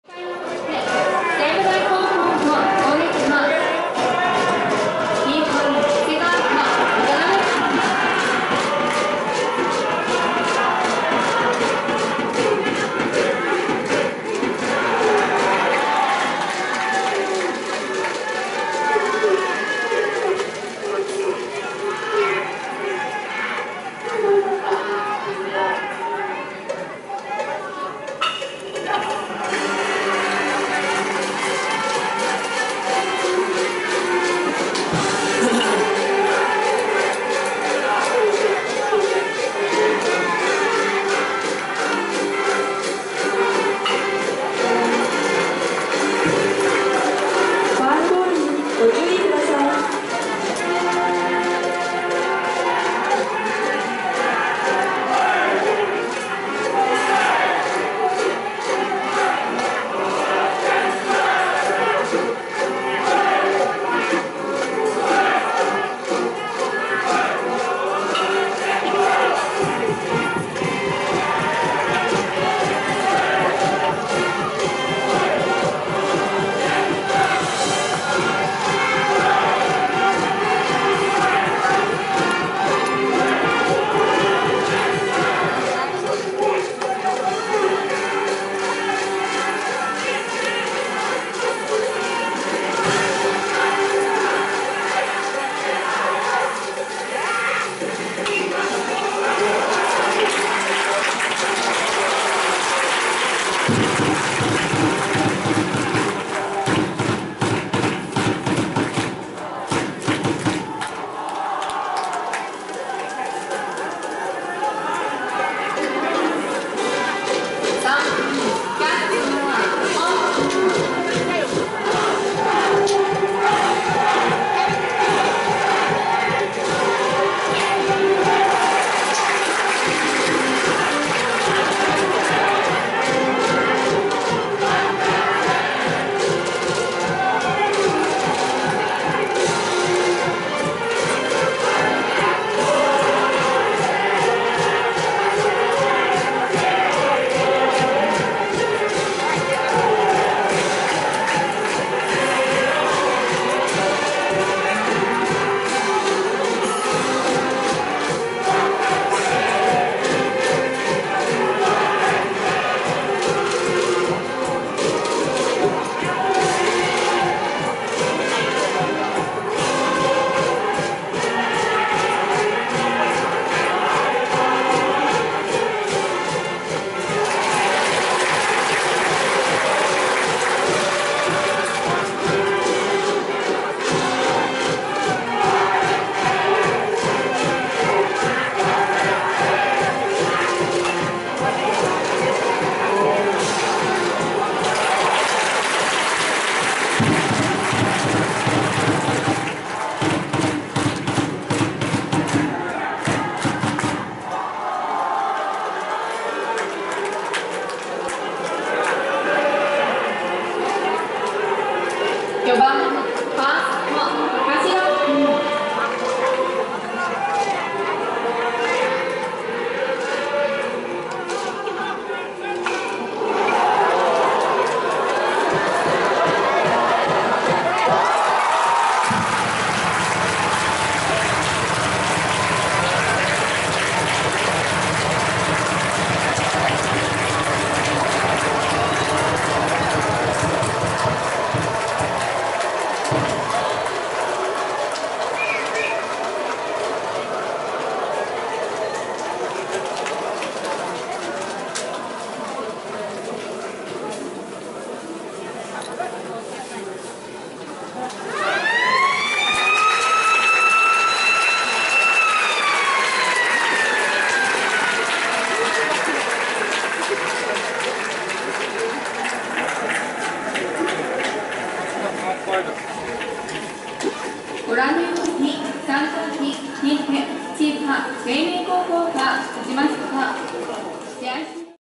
点差はわずかに1点！西武台の攻撃は2番からの好打順／決着の瞬間（2024秋季関東大会 西武台vs千葉黎明）
Japanese high school baseball
high school baseball